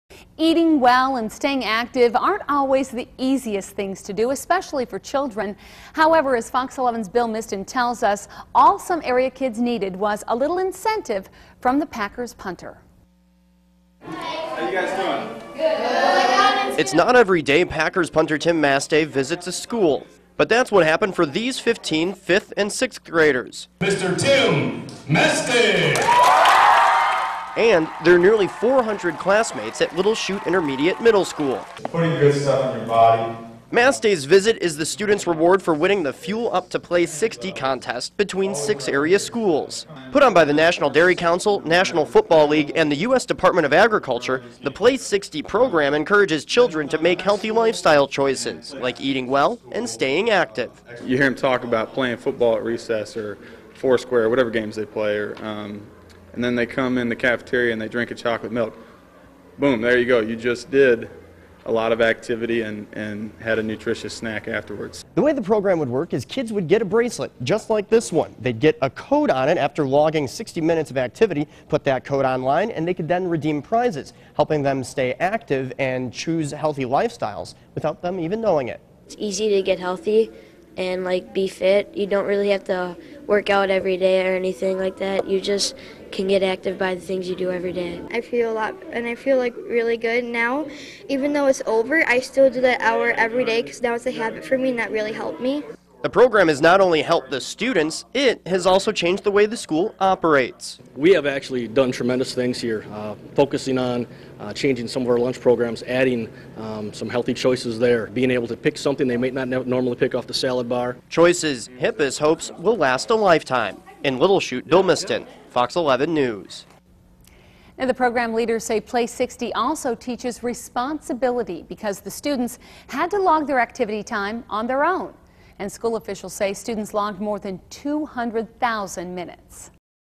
Students learn lesson in staying fit
It's not every day Packers punter Tim Masthay visits a school. But it didn't come without effort.